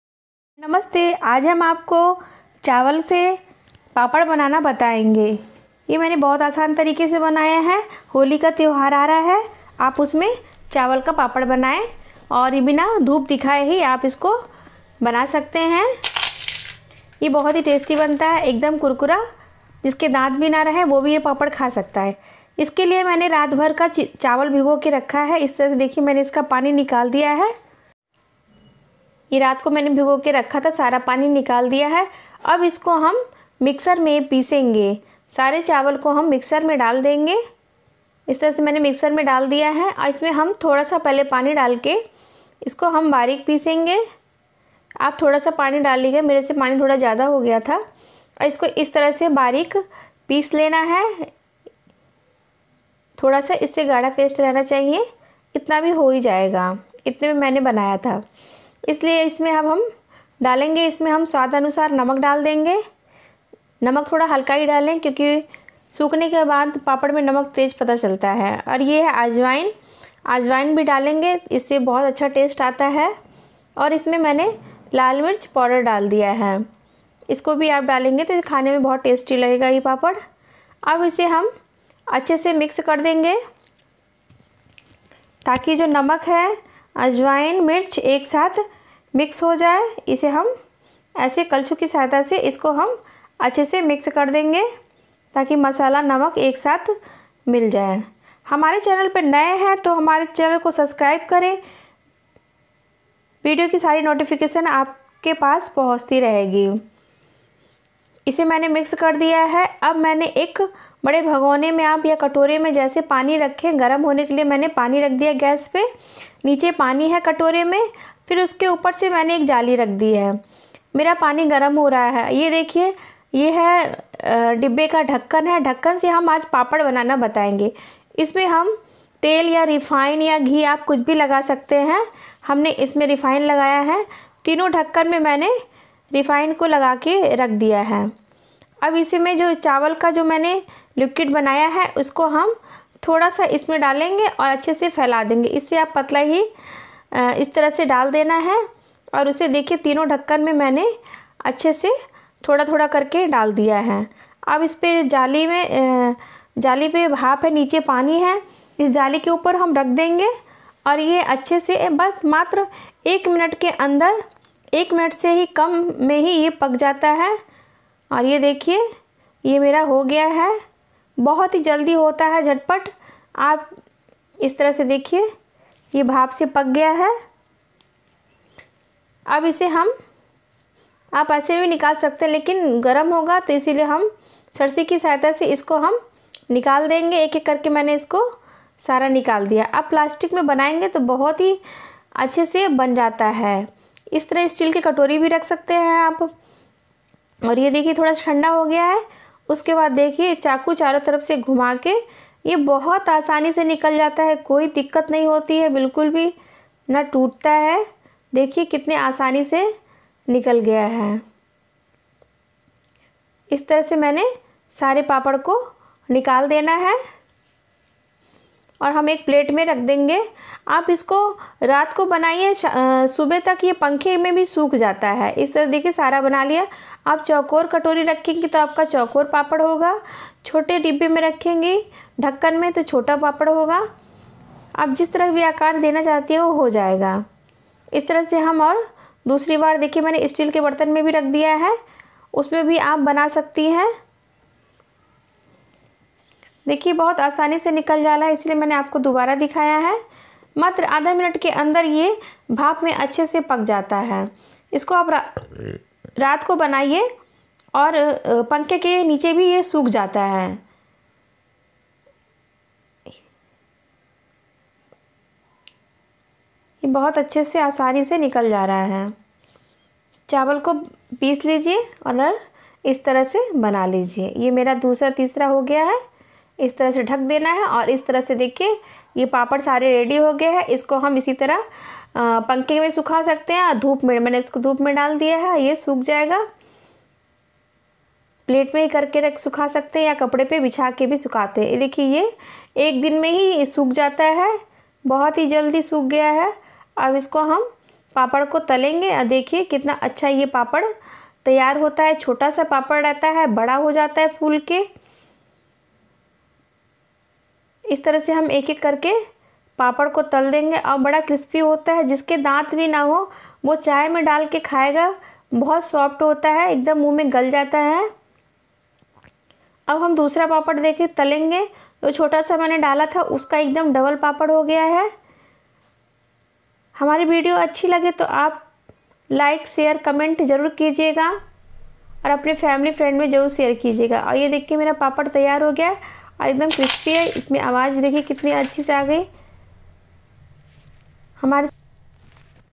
S Gunjan V, बिना धूप में सुखाएं हुए चावल का पापड़ बनाएं झटपट बनने वाला पापड़ chawal ke papad
हेलो दोस्तों मैं हूं गुंजन। मैं आपको खाने की रोज नई-नई और अच्छी रेसिपी बताऊंगी।